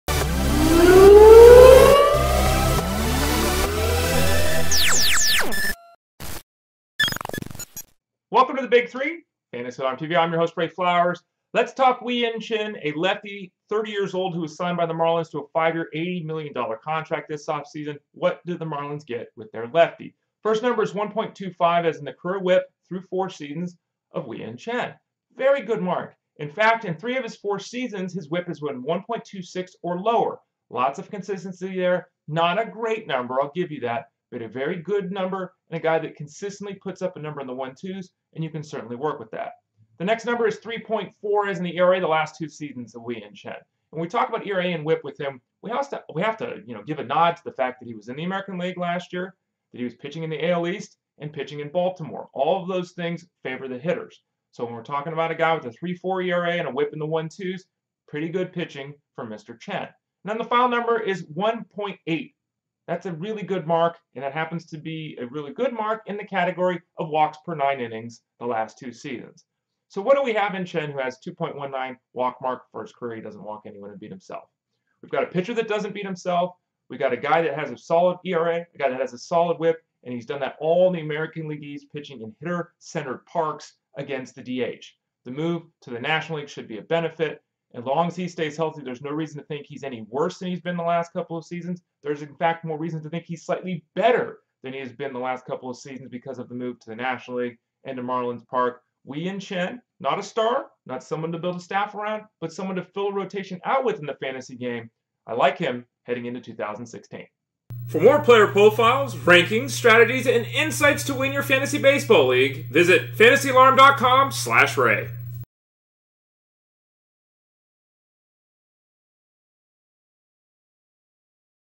Big 3 – Wei-Yin Chen moves from the AL to NL
Ray Flowers goes over the fantasy value of Wei-Yin Chen.